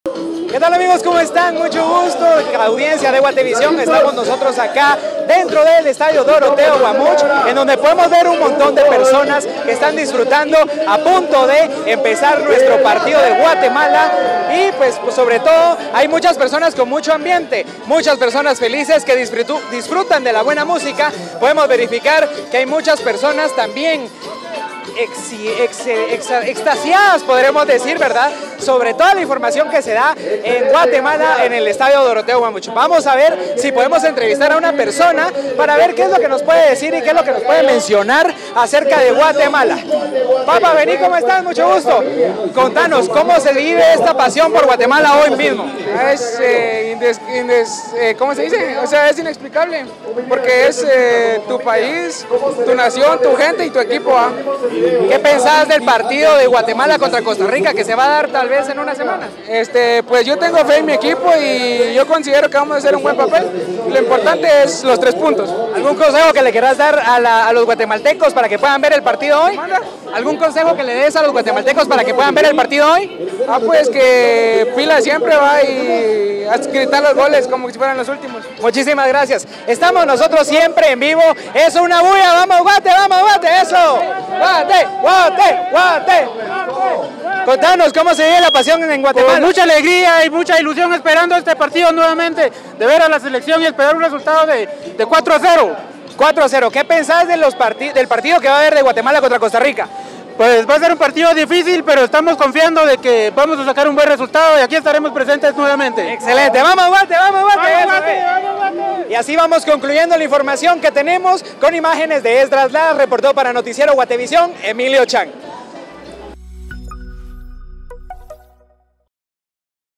Guatemala arranca el sueño en la Liga de Naciones de la Concacaf
La Selección Nacional de Guatemala inicia en la Liga de Naciones de la Concacaf, y el primer duelo es contra Martinica.
Aficionados se muestran optimistas con el papel de la azul y blanco, previo a la eliminatoria rumbo al Mundial de 2026.

🔔Más información🔔